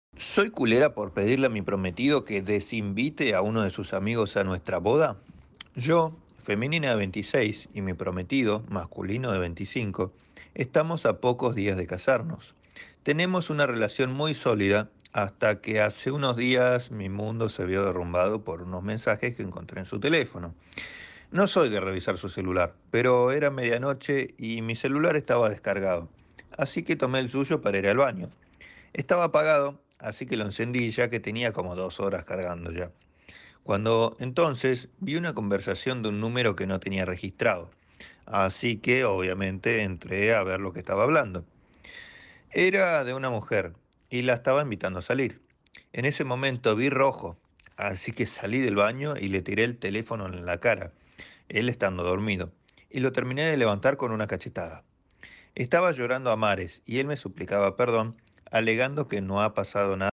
Historias de reditt: ¿Soy culera por querer que mi prometido desinvite a su amigo de nuestra boda?
Hola, ¿cómo estás?
Subo contenido entre todos los días de la semana, incluidos directos, espero que te guste :)